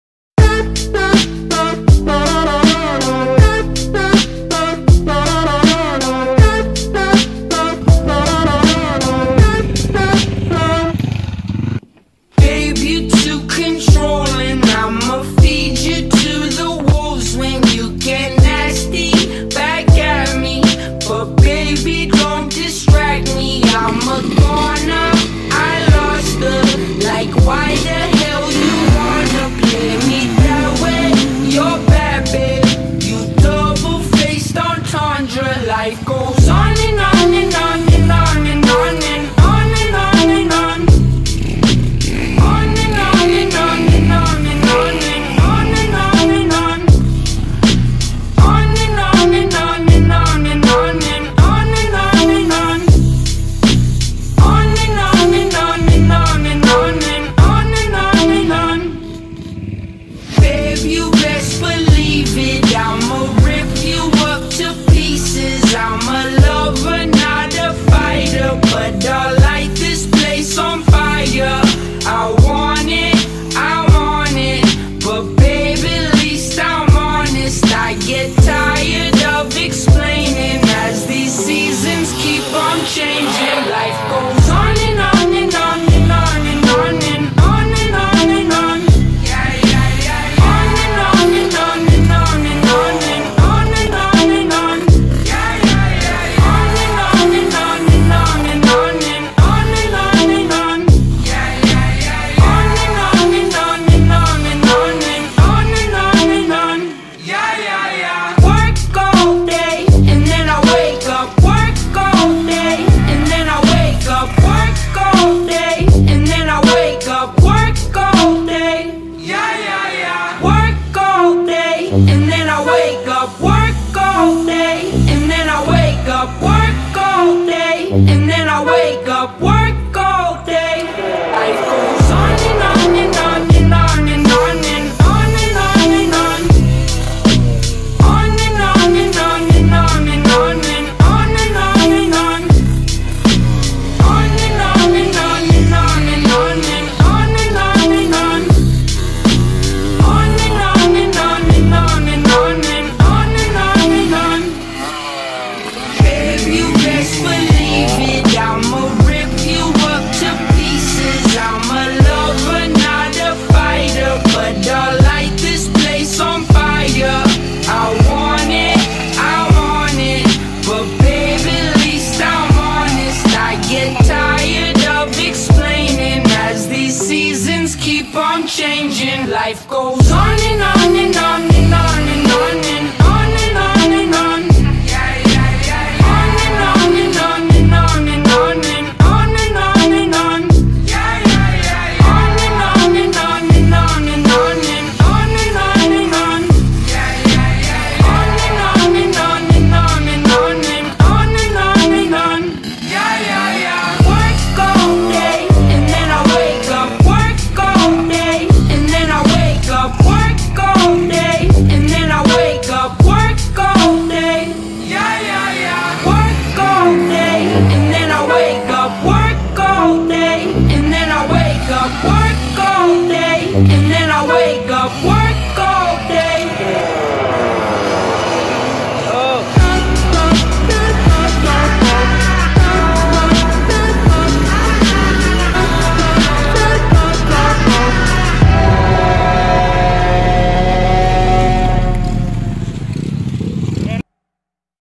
Derrick Virtudazo ft. Yzmx 200 (moto-x practice)
Rider: Engr. Derrick Virtudazo, 47 Years Old
Executive Rider in Bohol Motocross Race

Bike: Kawasaki Kmx125 engine upgrade to 200cc by Waway Racing, with YZ125 Rolling Chassi

Racetrack: Huwa - Huwa Racetrack @ Garcia Hernandez Bohol

Camera: Huawei Nova 5t Smart Phone
Edit By: Capcut